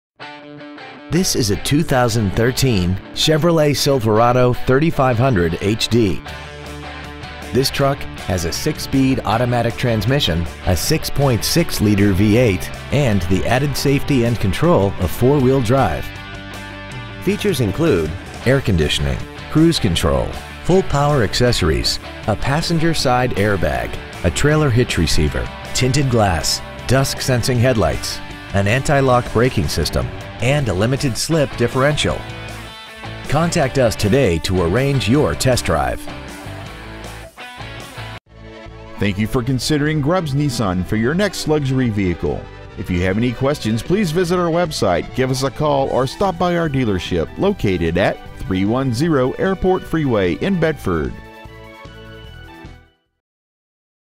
Used 2013 Chevrolet Silverado 3500HD Ft.Worth TX
We have been honored to serve the Bedford TX area , we promise that your experience at our dealership will exceed your expectations ! Year : 2013 Make : Chevrolet Model : Silverado 3500HD Engine : 6.6L V8 Diesel Trans . : Automatic 6-Speed Exterior : Black Miles : 81,537 Interior : Dark Titanium Stock : N10840A Grubbs Nissan Airport Freeway Bedford , TX 76022